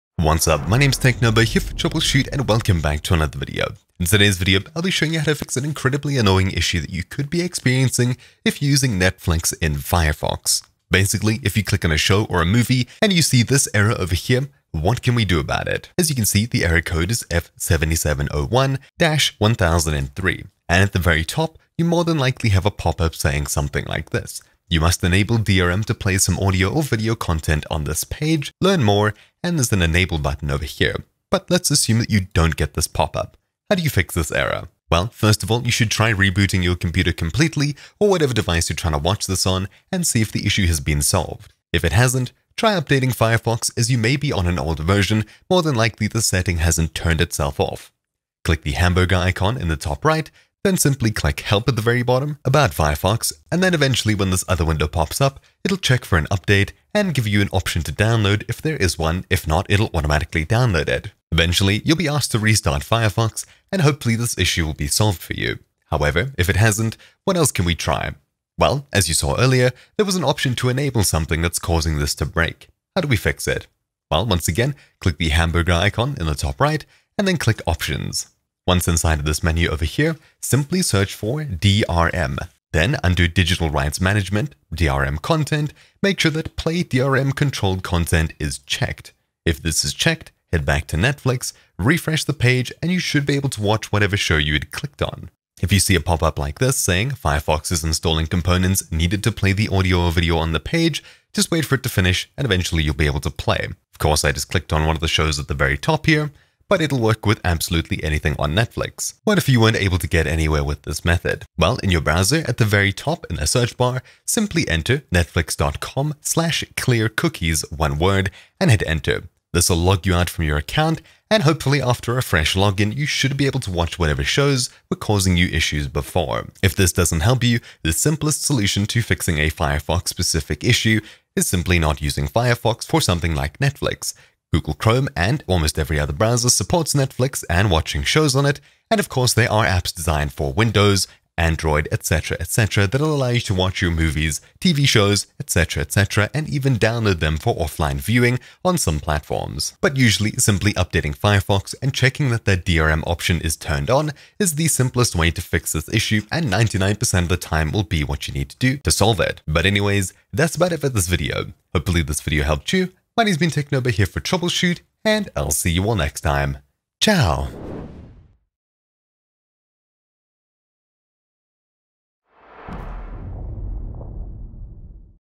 Fix Netflix F7701-1003 Error | Easy Firefox Solutions Full Guide
Seeing Error F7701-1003 or "You must enable DRM to play some audio or video on this page"? Well, this is a DRM issue with Firefox. This video shows you some solutions to get Netflix to work properly again. By the end, you should be able to watch your favourite shows and movies again.

Timestamps:
0:00 - Explanation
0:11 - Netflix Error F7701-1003
0:34 - Reboot
0:41 - Updating Firefox
1:05 - Enabling DRM controlled content
1:49 - Clearing cookies
2:06 - Use another browser